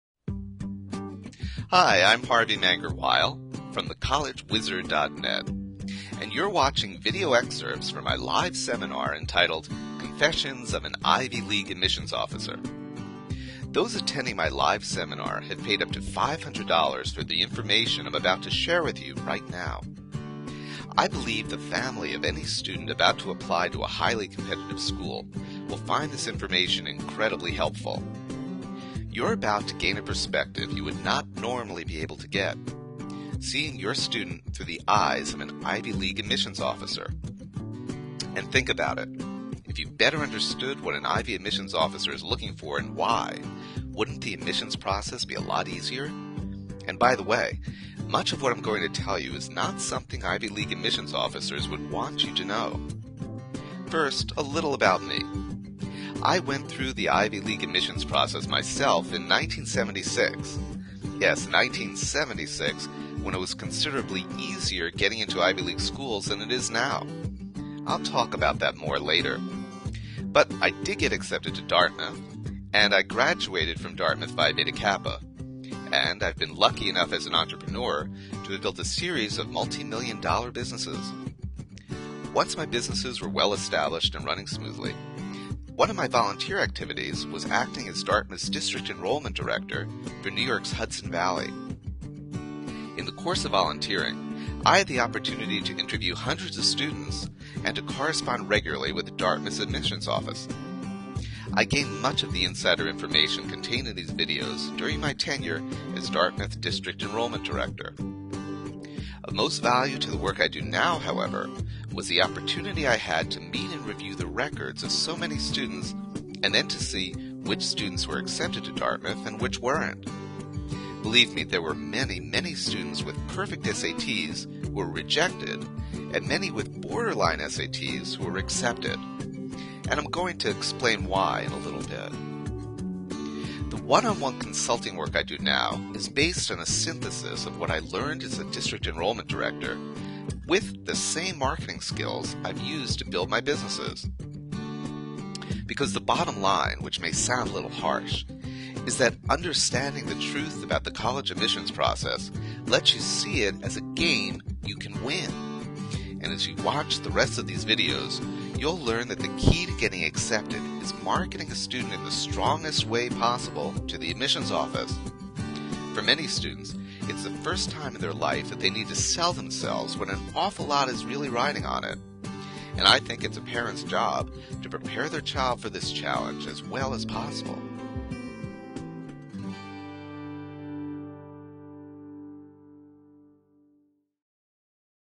Harvey Manger-Weil reveals the secrets of college admissions
Harvey Manger-Weil, The College Wizard, explains the secrets to getting into America's toughest colleges.  Harvey Manger-Weil is America's leading SAT tutor.  95% of the students he coaches gain admission to their first choice school.